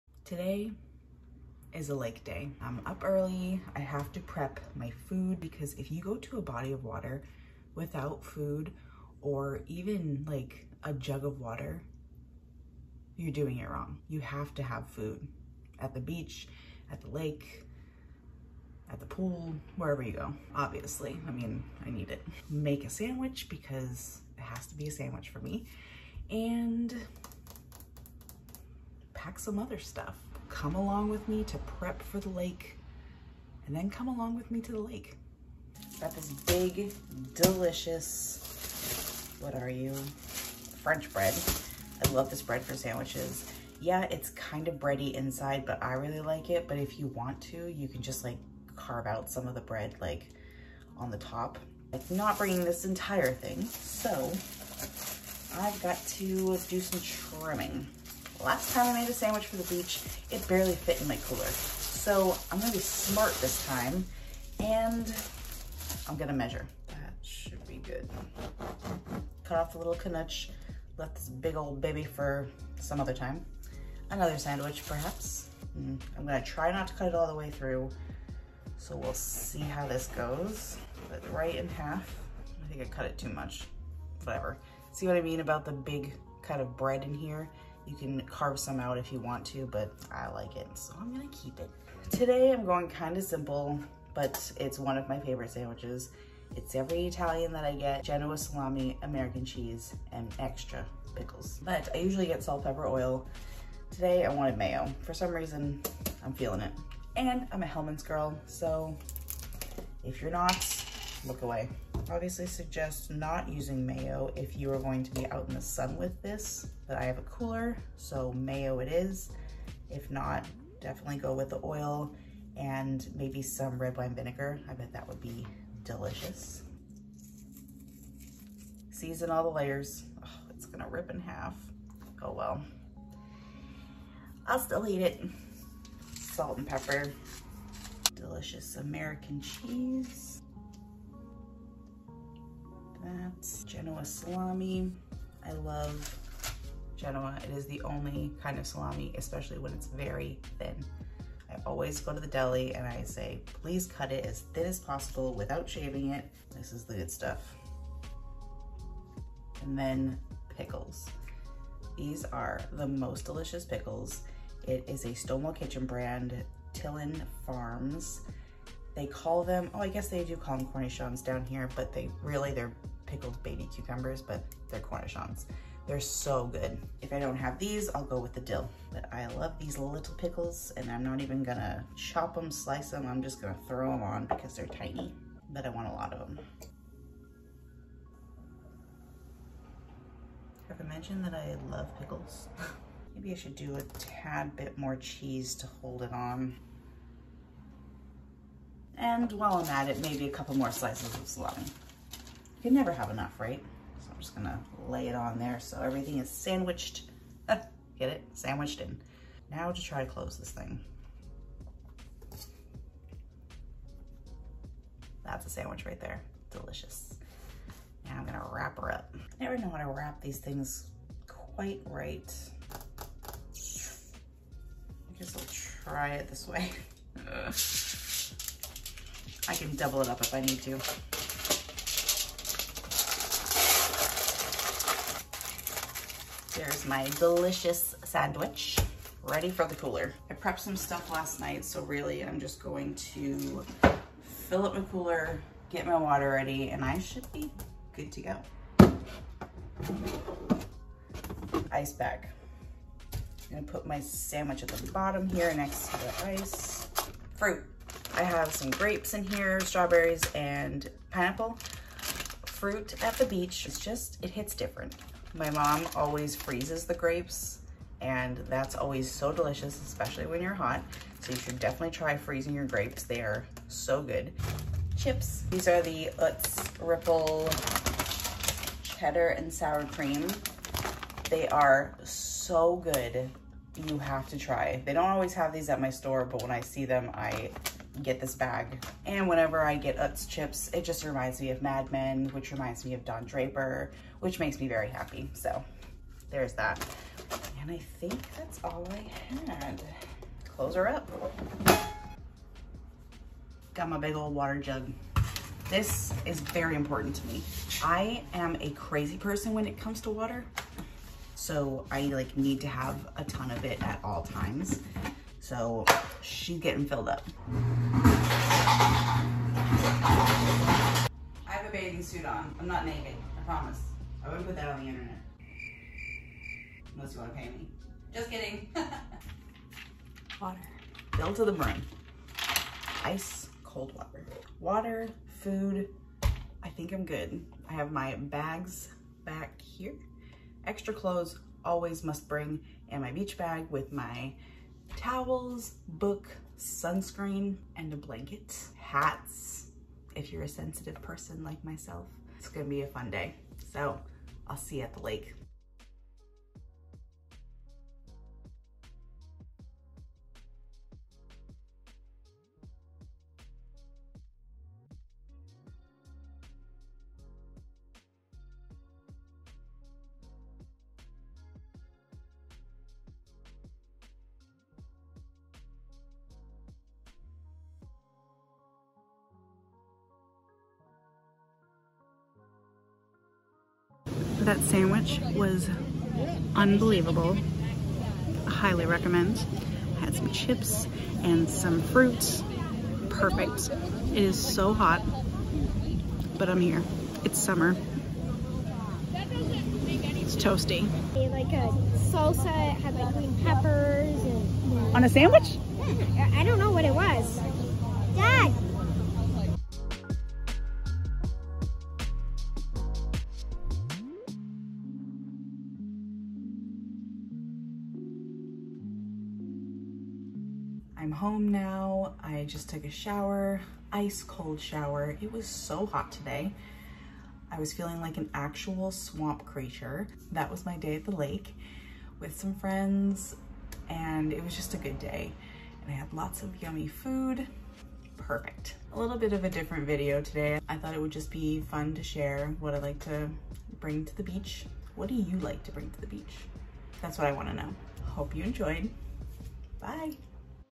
COME TO THE LAKE WITH ME | BEACH EATS
Come along to the lake with me for the day! I'll pack all my snacks and get out to the water. I absolutely have to have food and drinks when I go to the beach. If you don't... sorry, but who hurt you?

Today I made a genoa, cheese, and extra pickle on a yummy french loaf, brought some fruit, and of course some chips. It was the perfect amount of food and it kept me full all day long!